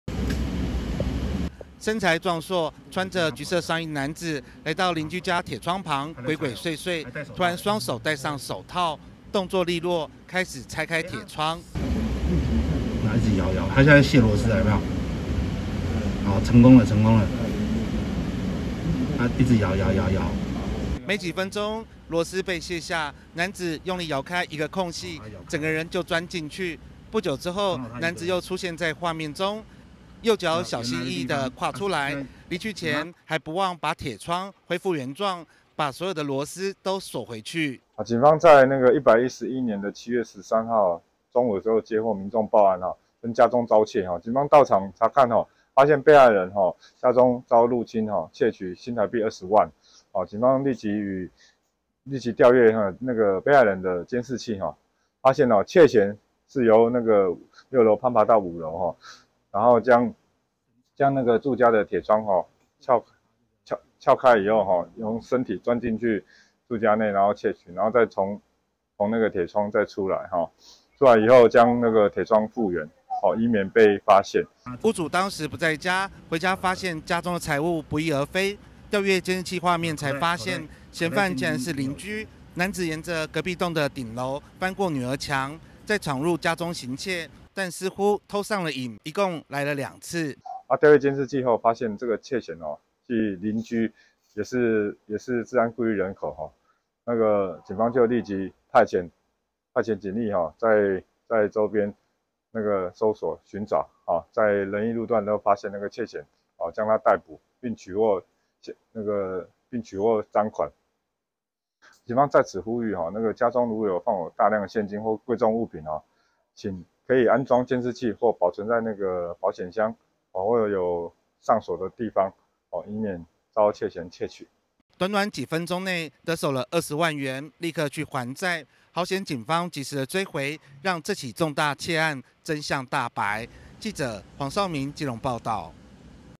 竊盜案｜偷東西｜鄰居竟然是小偷 拆窗戶闖民宅偷20萬
(中嘉新聞記者黃紹銘/基隆採訪報導) 真的是很大膽，歪腦筋動到鄰居身上，基隆一名屋主發現家中20萬現金不翼而飛，調閱監視器才發現，竊賊居然是鄰居。